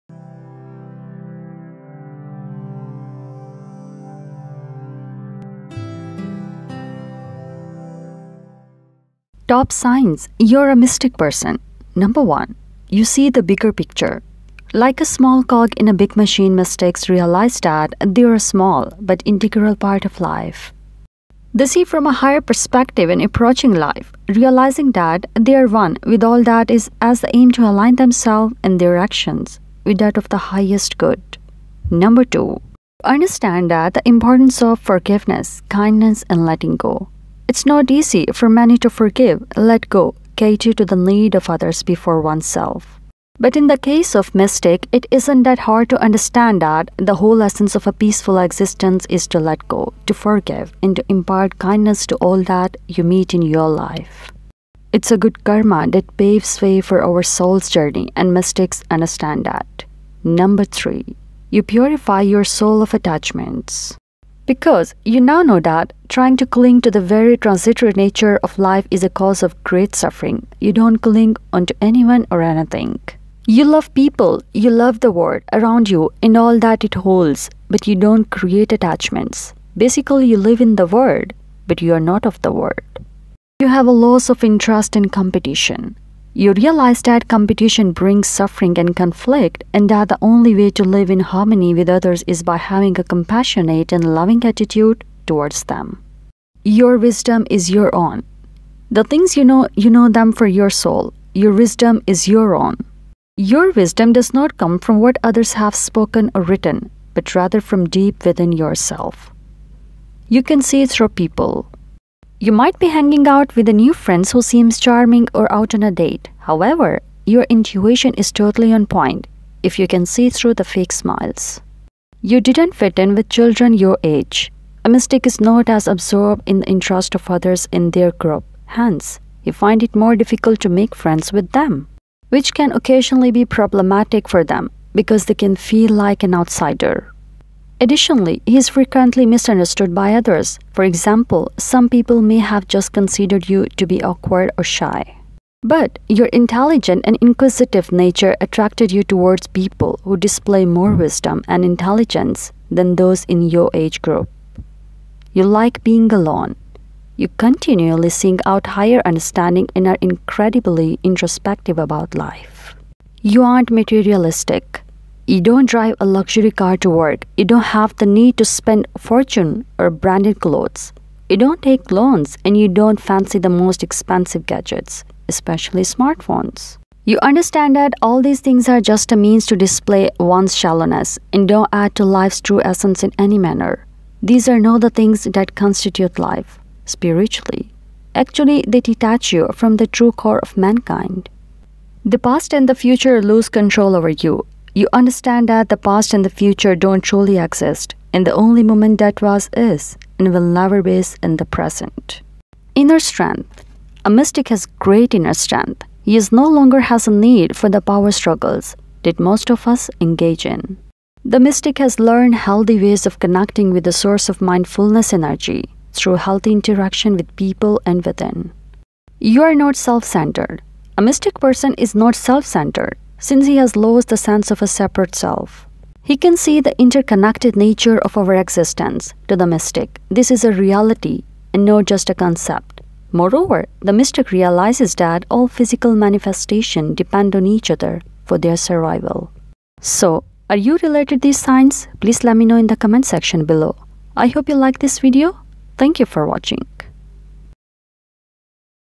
Top 12 Signs You Are "Mystic Person"
in this video I have mentioned the top 12 signs you have a mystic personality.
or you are mystic person